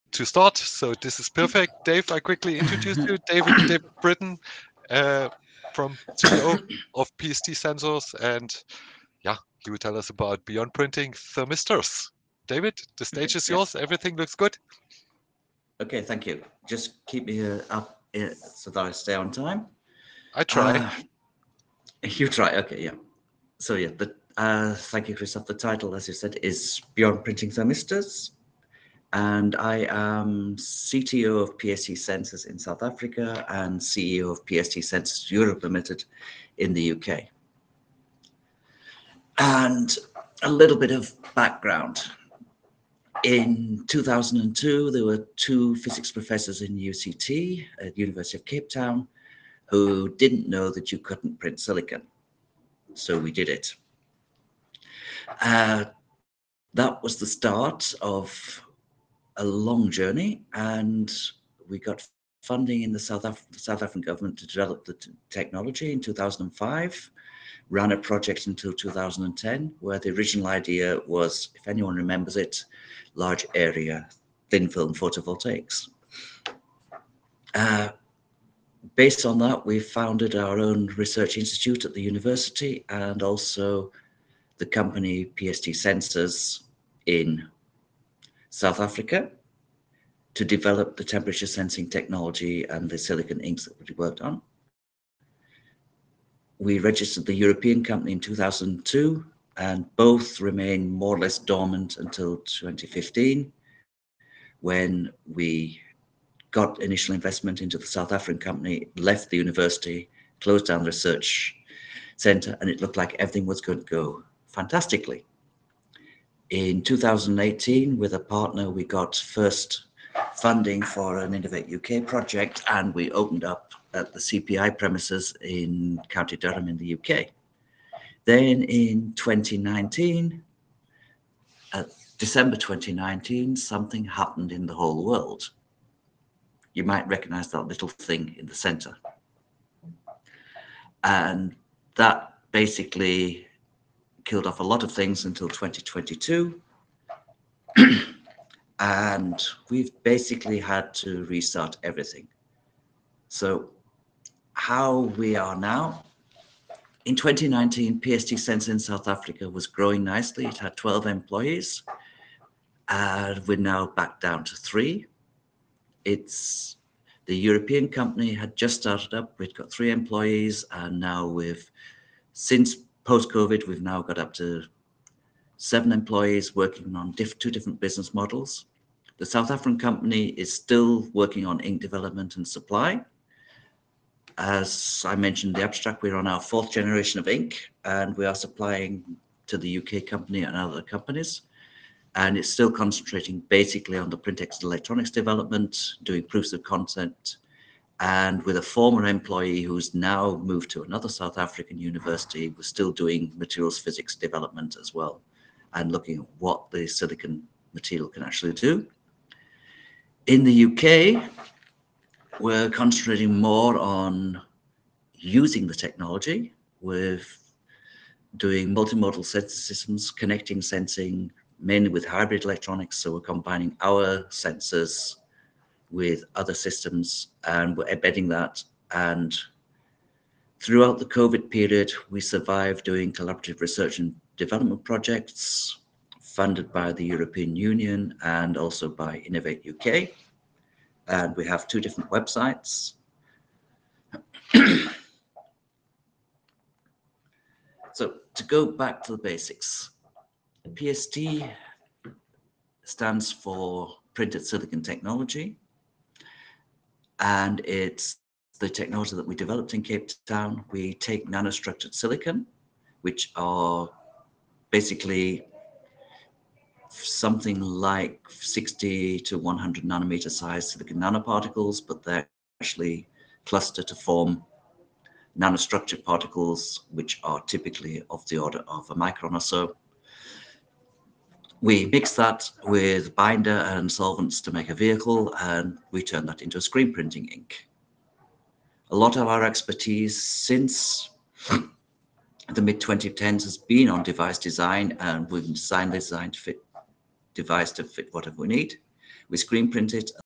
PST Sensors | Beyond Printing Thermistors
David Britton, Director

Just over a decade ago, PST Sensors in South Africa introduced its first printed temperature sensor – a simple NTC thermistor printed on paper using its first-generation semiconducting silicon ink. In 2023, PST Sensors Europe in the UK is now implementing PST’s fourth generation sensor ink in EV batteries, agrifoodtech and healthtech. The latest generation of inks exhibit improved adhesion to a wide range of substrates - including polyimide, aramid and, polycarbonate – as well excellent stability which, when combined with internet connectivity and real-time predictive analysis, enables all of the applications which will be discussed.